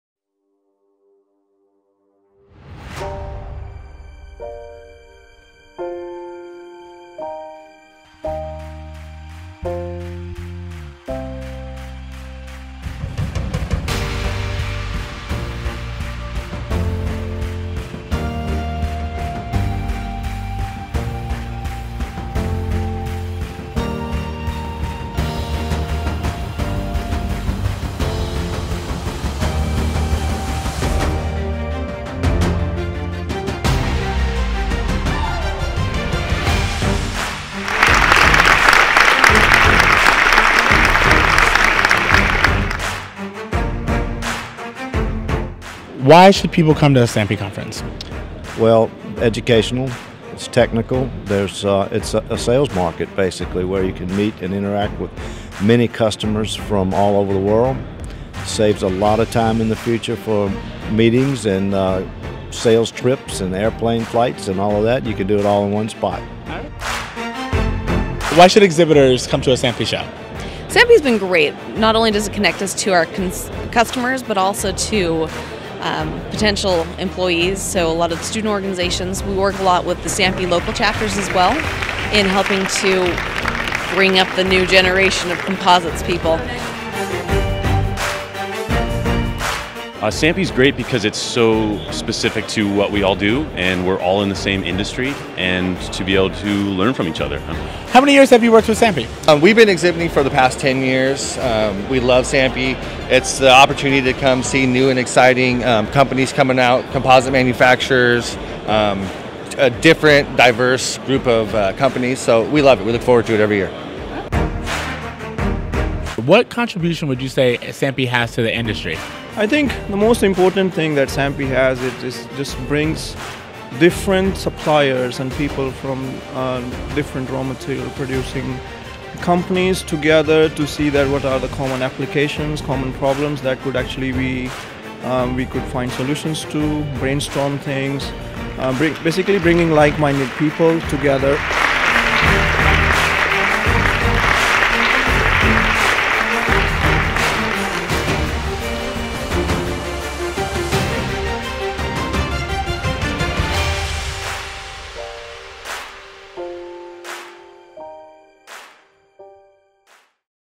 SAMPE C&E 2017: Success in Seattle
Highlights from SAMPE's 2017 Annual conference in Seattle.

SAMPE Seattle is the only technical conference and exhibition that is exclusively focused on advanced materials and processes. This event connects you with emerging innovations, business solutions, and proven technology advancements.